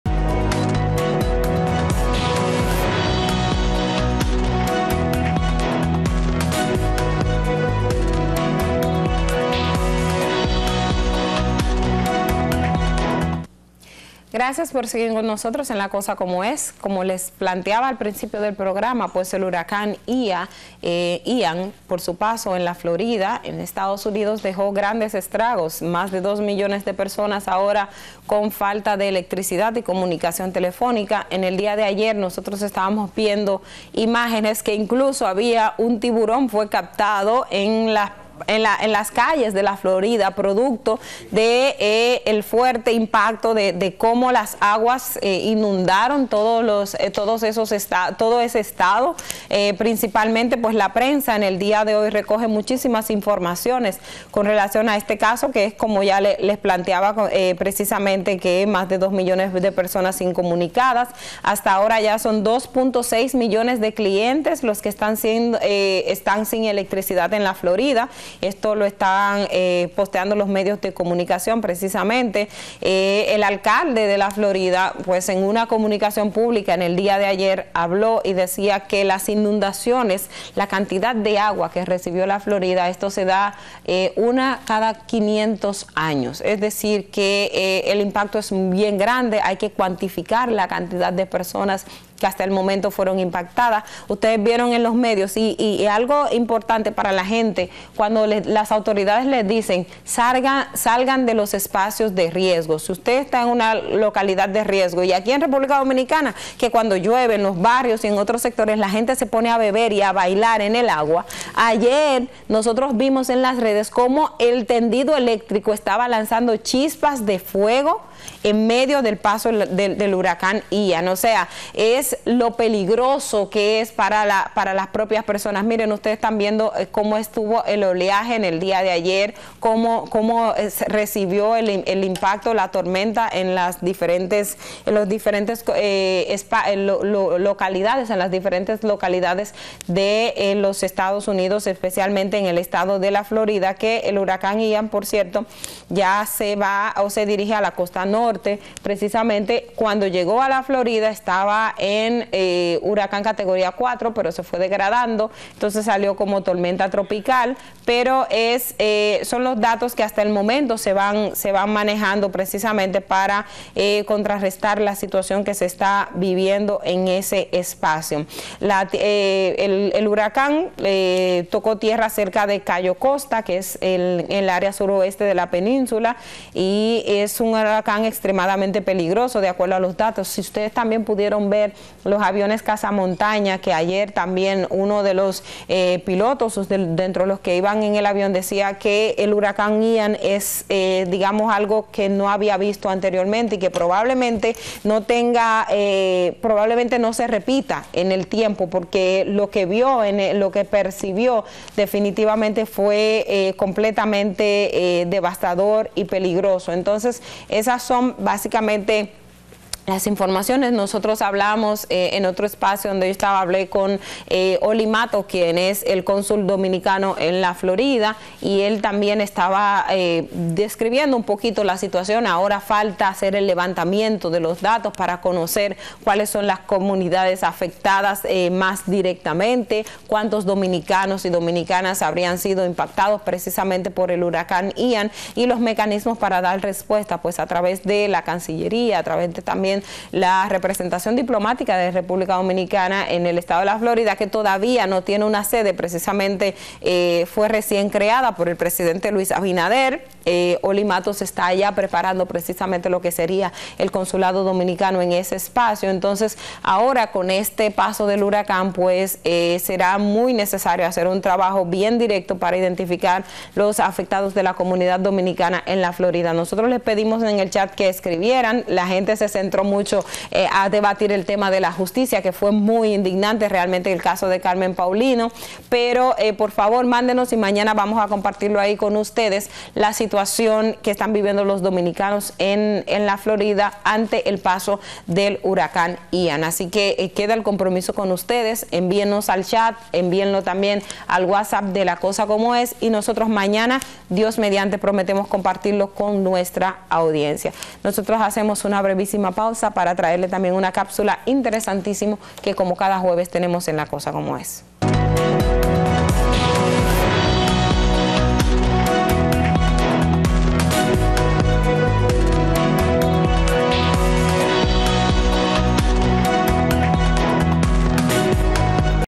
Huracán Ian deja catastróficas inundaciones en costa de Florida
Canal Oficial Periodista Edith Febles